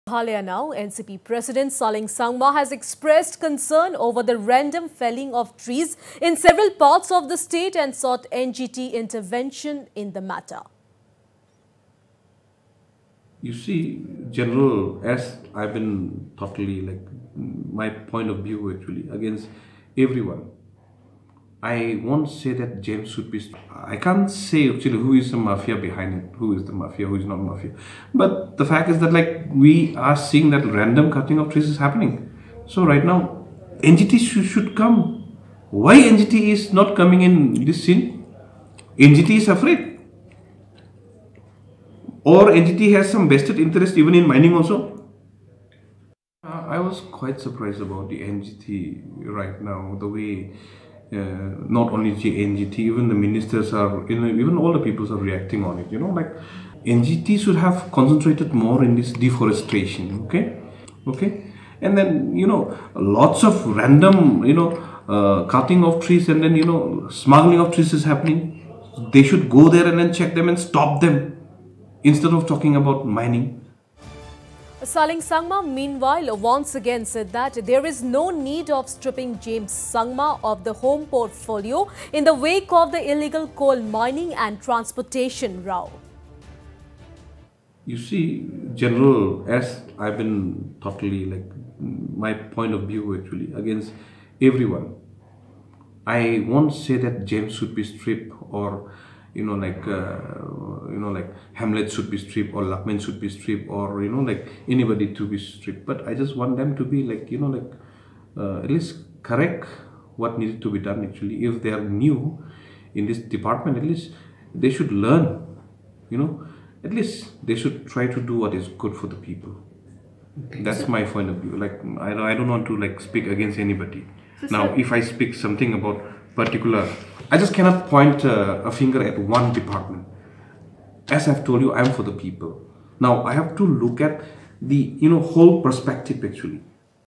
Meghalaya NCP seeks NGT intervention over tree felling in several parts of state
Meghalaya NCP president Saleng Sangma has expressed concern over the random felling of trees in several parts of the state and sought NGT intervention in the matter.
Saleng Sangma meanwhile, once again said there is no need of stripping James Sangma of the Home Portfolio in the wake of the illegal coal mining and transportation row.